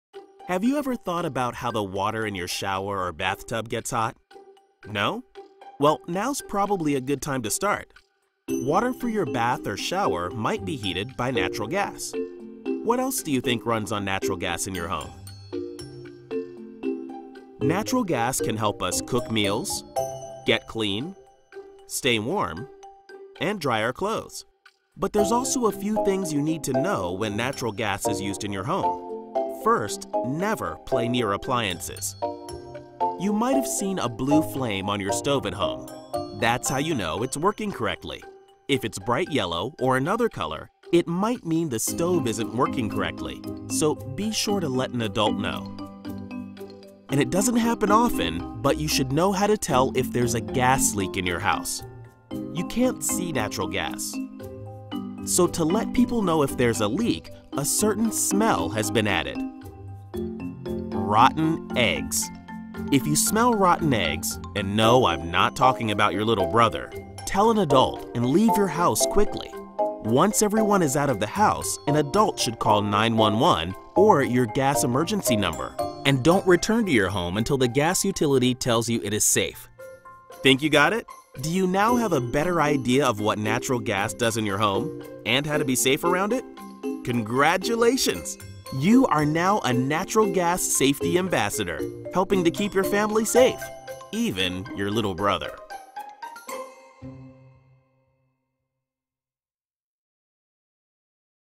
Natural Gas Safety for Kids Presented by AGA
AGA created this video to help educate youth about the importance of natural gas safety in the home. The animated video, aimed at elementary and middle school aged children, explains which appliances use natural gas, how to identify a natural gas leak and what to do if a leak occurs. These key safety messages are not only critical to family and caregivers, but the next generation of utility customers.